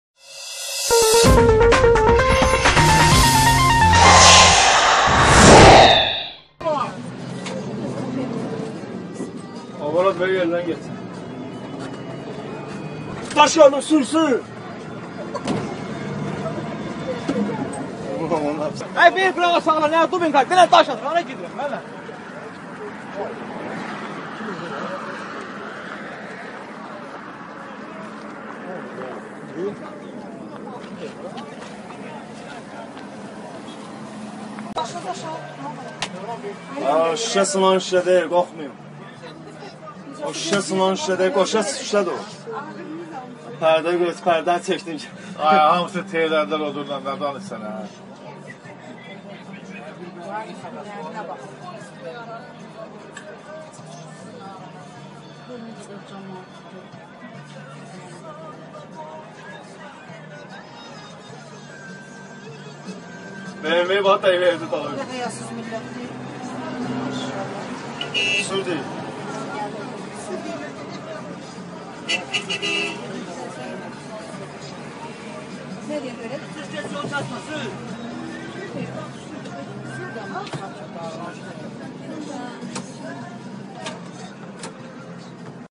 Gəncə-Bakı yolunda aksiya...
AzadlıqRadiosuna göndərilən videoda görünür ki, bir qrup şəxs avtomobil yolunu bağlayıb. Hadisənin Gəncə-Bakı magistral avtomobil yolunda baş verdiyi deyilir. Etirazçıların kim olduqları və nəyə etiraz etdikləri dəqiq məlum deyil. Amma bildirilir ki, yolu kəsənlər alüminium zavodunun yaxınlığında yaşayan məcburi köçkünlərdir. Onlar güclü külək zamanı zavoddan qalxan tozun ətrafa yayılmasına etiraz edirlər.